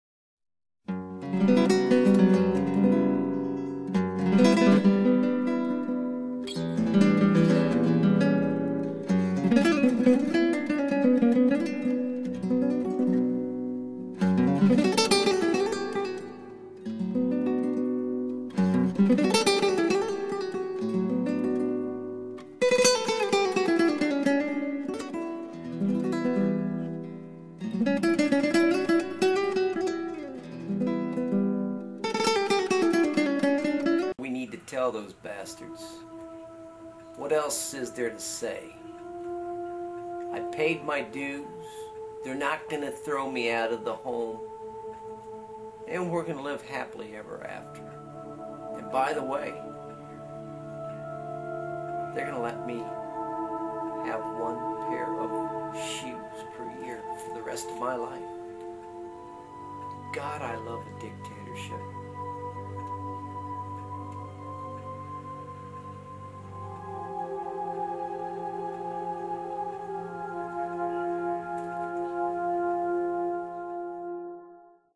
Socialist, HD, killer and mafia
Fictional short movie using a Pansonic AG DVC20p, and Windows Movie Maker editing software.